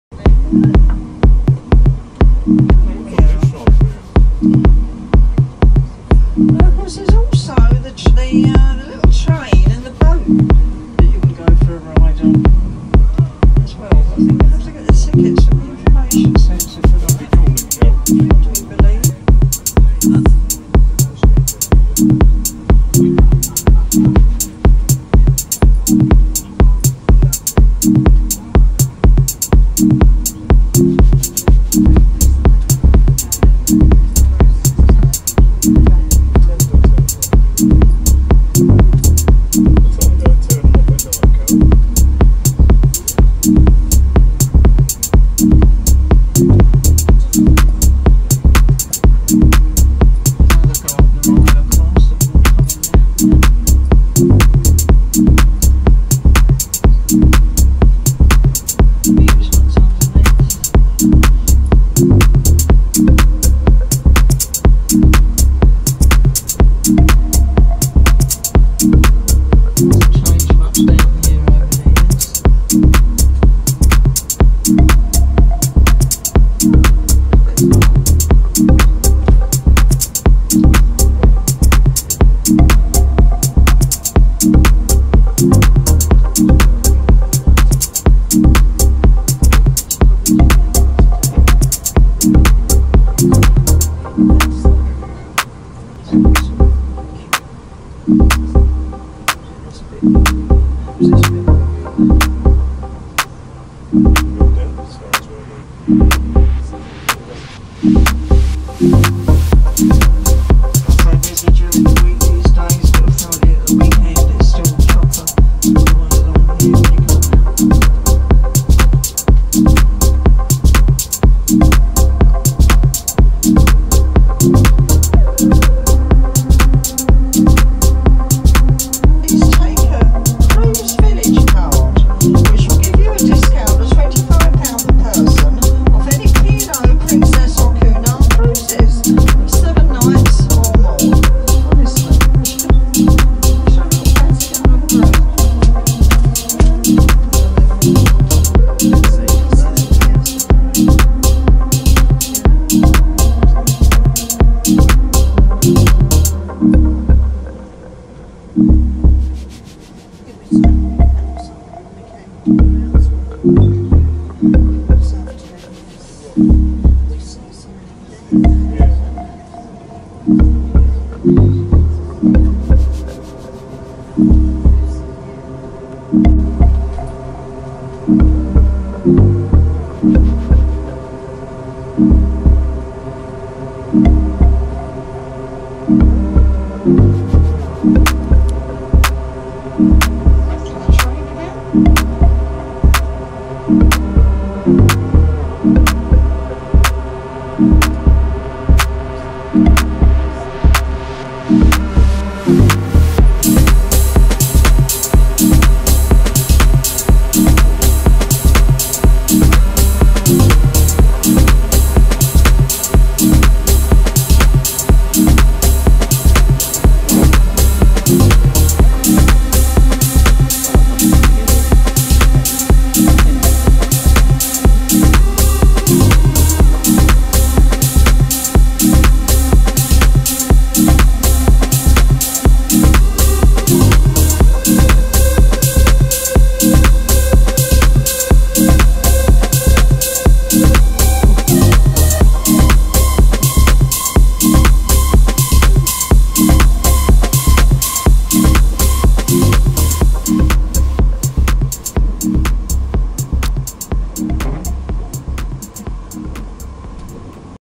Blackpool Illuminations 2017
Blackpool şehrinin ışıkları.. Her sene Eylül ayında gerçekleşiyor...